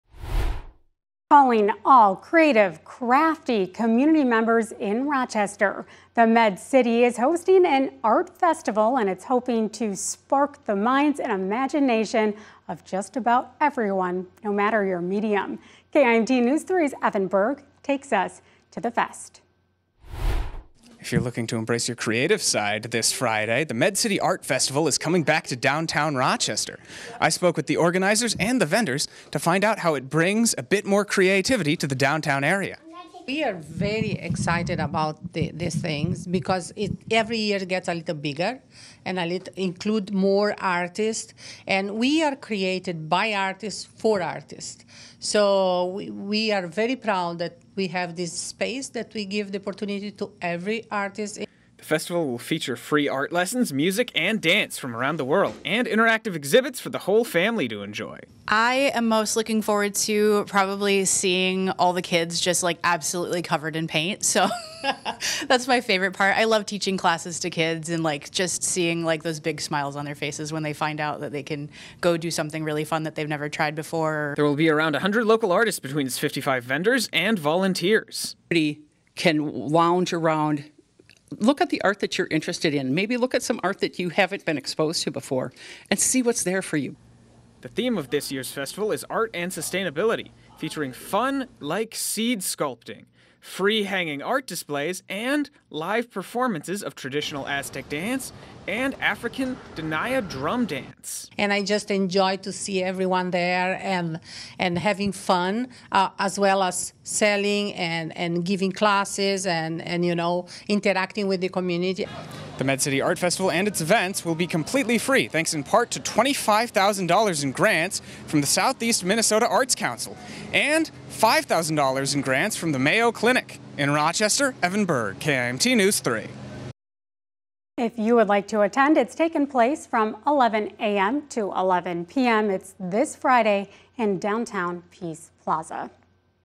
Med City Art Festival is coming to town this Friday
KIMT News Three's Evan Berg spoke with organizers in it's goal to make art more inclusive in the Med City area.